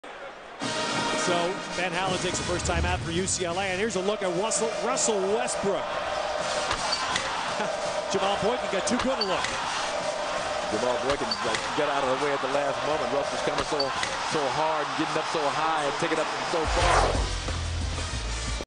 Westbrook dunks over Jamal Boykin
UCLA sophomore guard Russell Westbrook dunks over Jamal Boykin of the Cal Bears early in the first half 1/5/08